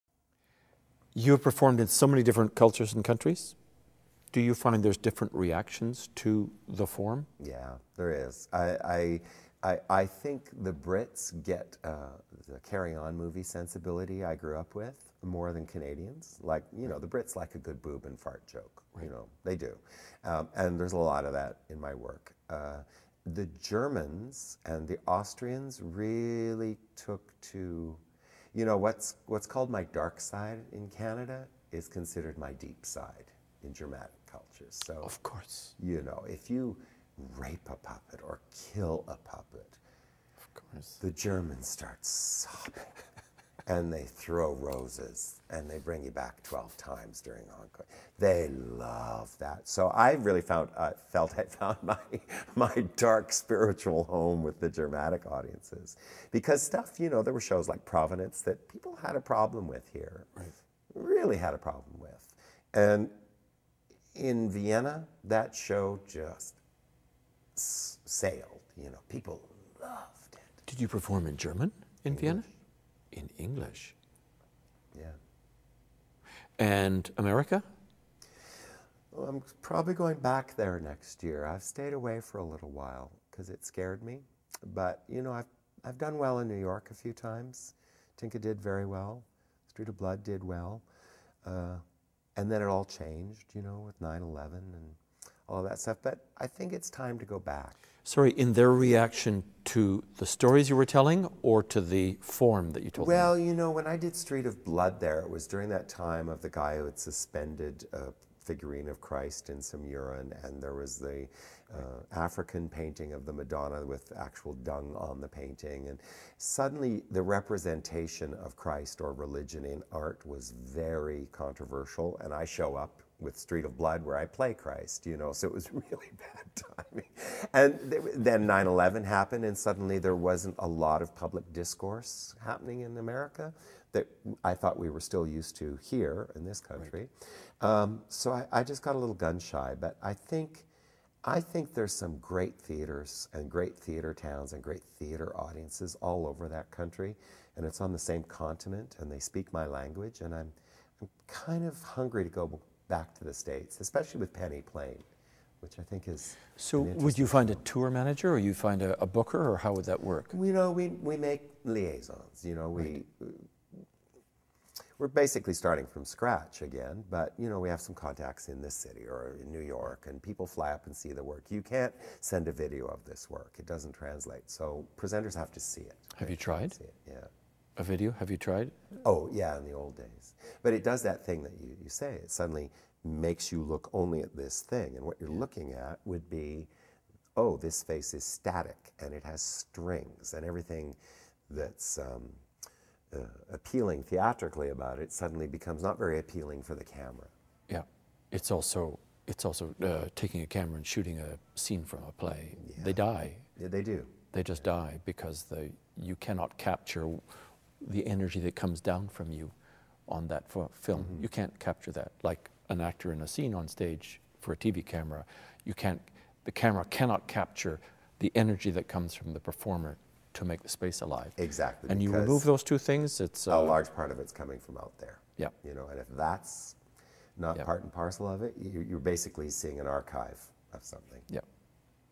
Ronnie Burkett on puppetry in different cultures (Part 16 of 20)
“what’s considered my dark side in Canada is considered my deep side in Germanic countries”

Ronnie Burkett discusses the different reactions to his work in different cultures and countries.

One of Canada’s foremost theatre artists, Ronnie Burkett has been credited with creating some of the world’s most elaborate and provocative puppetry.  In this series of interviews with Canadian actor RH Thomson, Burkett talks about characters, storytelling, process and more.

In 2001 Ronnie Burkett received the President's Award from the Puppeteers of America, and in 2003 the Herbert Whittaker Award for Outstanding Contribution to Canadian Theatre. In 2009 he was awarded the Siminovitch Prize in Theatre for Design. In June 2019, he was named Officer of the Order of Canada.

Interviewed by R.H. Thomson on February 7th, 2012 in Toronto.

Filming location courtesy of Factory Theatre.